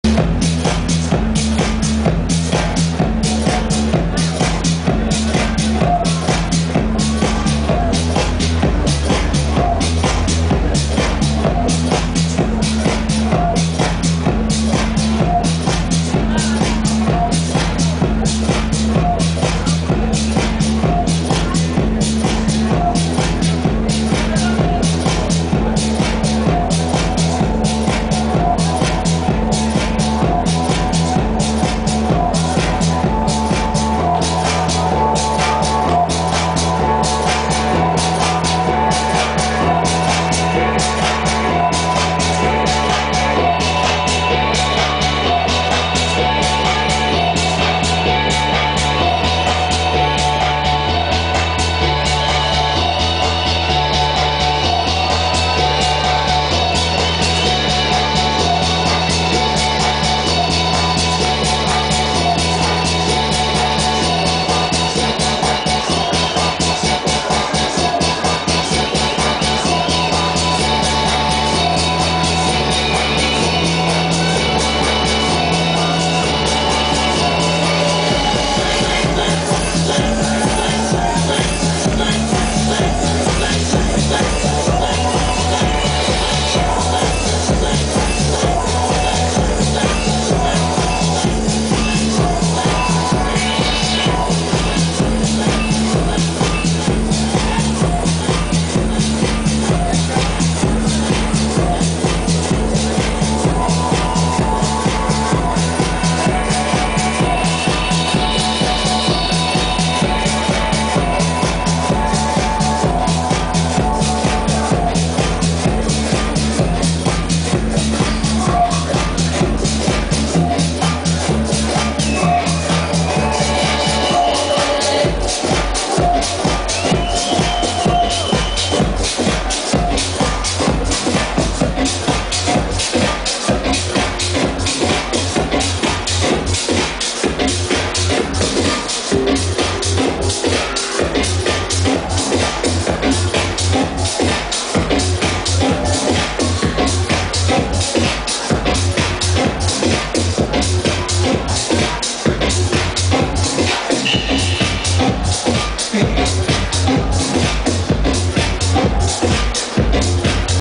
AudioJack-NaturaFestival-Feb.6.2011-LaMarquesa-Mex.Video1.MPG
"AudioJack" (UK) in Natura Festival (Open Air) in La Marquesa-Mexico on February 6,2011.
Event By: "Tribal Vision Mexico and Maia Records"
Video No.1 Recorded By: Gely González(DJG-Mind)from Ce.TV / Femine Sessions & Magazine / G-Mind Productions Mx.